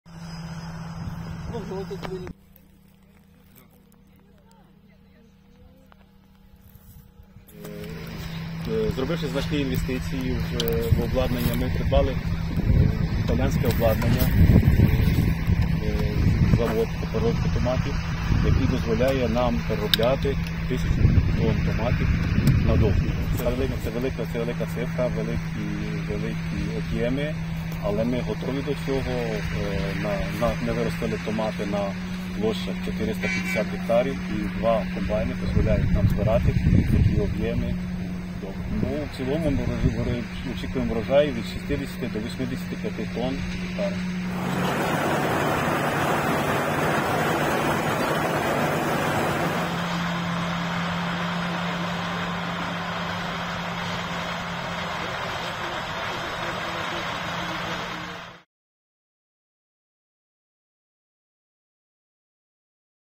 На Одещині почали збирати врожай томатів
Вирощують помідори  на крапельній системі поливу на площі 450 гектарів.

Близько тисячі тонн вдається зібрати за добу.

Для Одещини це перший такий досвід, коли томати збирають у великих об’ємах для подальшого виробництва соків та томатного пюре

В цілому аграрії очікують врожай від 60 до 85 тонн з одного гектара.

Відео: Укрінформ
Автори: зйомка - Ніна Ляшонок, монтаж - Роман Храбров